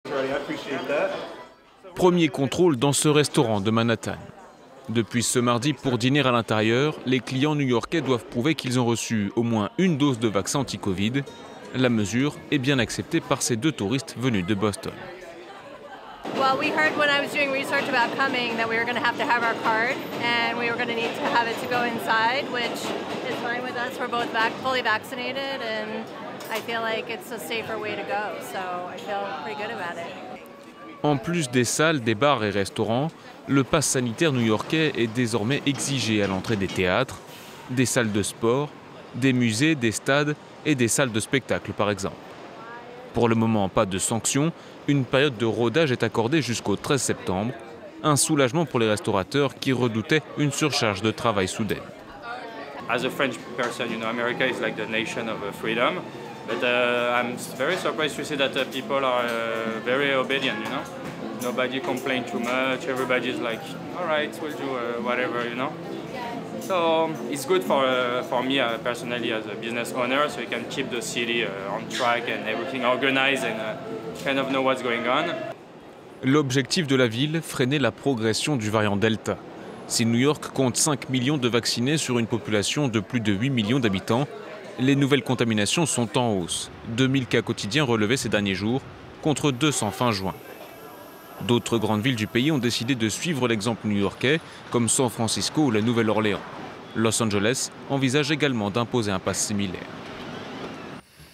Pandémie de Covid-19 aux Etats-Unis : la ville de New York adopte le passe sanitaire • FRANCE 24
A New York depuis mardi 17 août, le passe sanitaire est le sésame pour de nombreuses activités à New York, une nouvelle règle qui s'installe en douceur, dans une ville où la pandémie a laissé des traces.